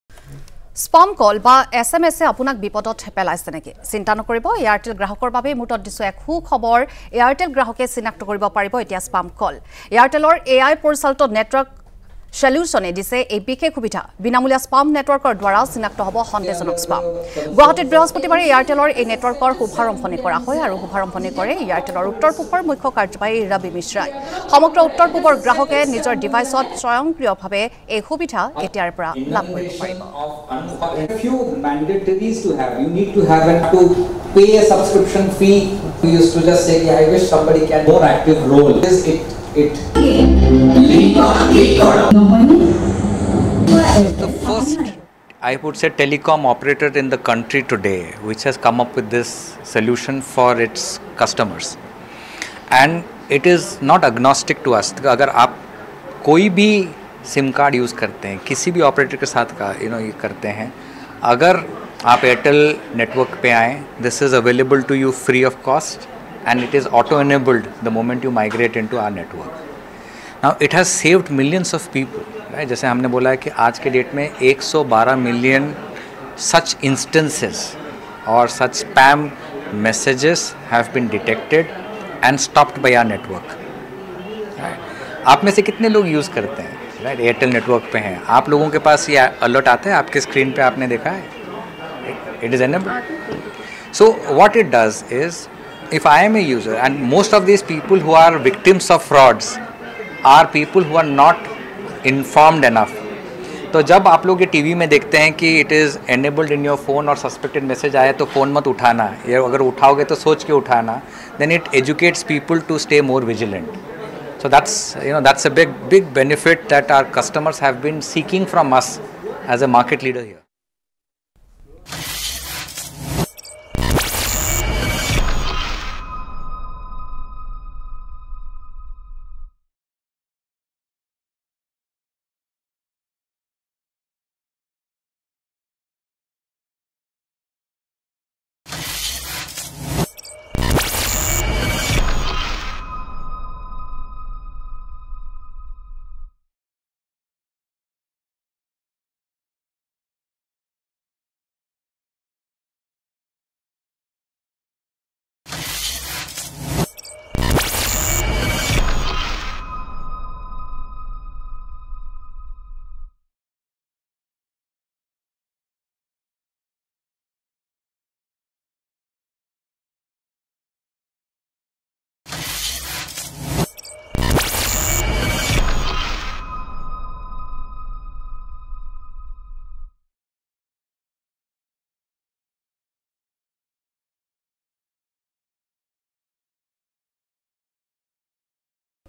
Spam কল বা SMS-এ আপোনাক বিপদত পেলাইছে নেকি?
চিন্তা নকৰিব, এয়াৰটেল গ্ৰাহকৰ বাবে এটা সু-খবৰ
এয়াৰটেল গ্ৰাহকে চিনাক্ত কৰিব পাৰিব Spam কল